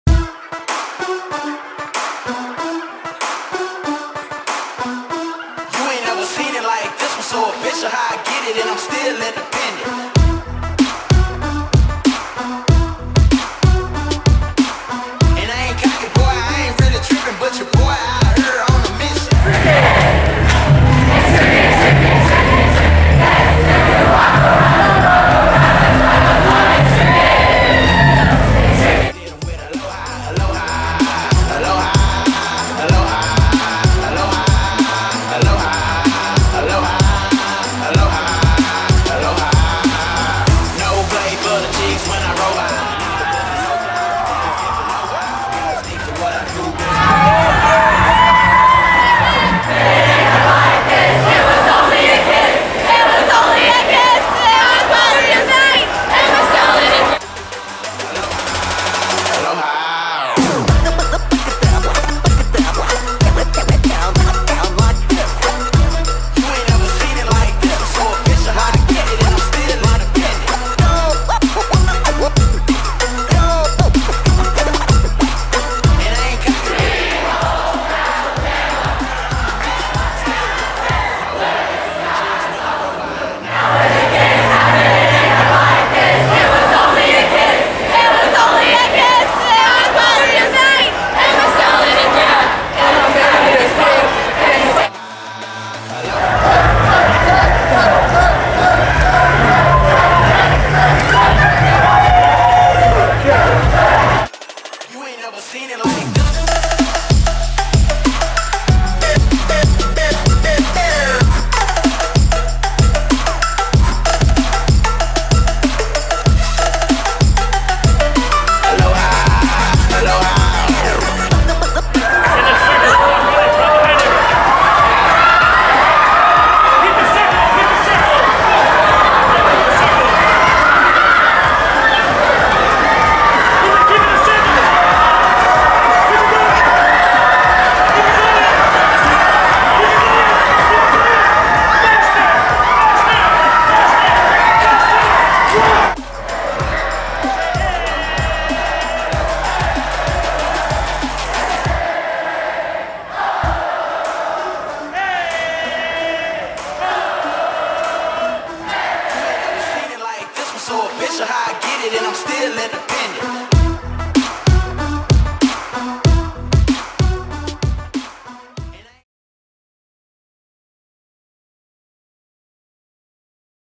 Austin City Limits ACL 2019 Silent Disco powered by: Quiet Events
For the 3rd year in a row Quiet Events drew out thousands to partake in the Silent Disco Show under the Titos Tent at ACL.  This two week 6-night event has over 14,000 people in and out in just 2 hours a night. This is also where Quiet Events started the "Silent Cyclone" making people run around in circles at the party (see at end of video)